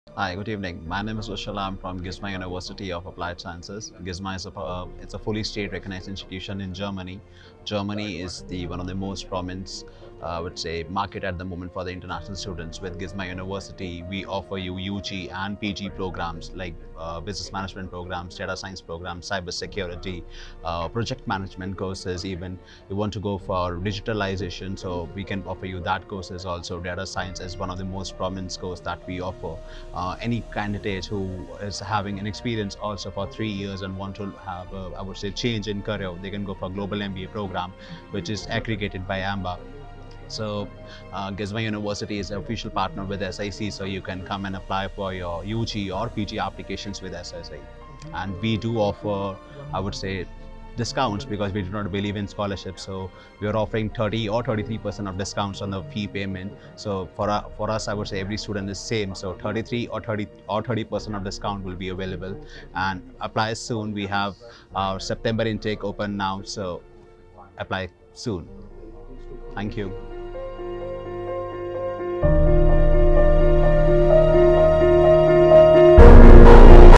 GISMA UNIVERSITY OF APPLIED SCIENCES | SIEC Education Pvt. ltd.
SIEC's Special Partner @gisma.business.school!

Here's the Business Development Manager from GISMA @rushil_gisma_gus, sharing his thoughts about Germany & GISMA!

Listen to what he has to say 👆🏻🇩🇪

Have questions about Germany? Or your abroad study journey?

Then your very own partner @siec.india is here😌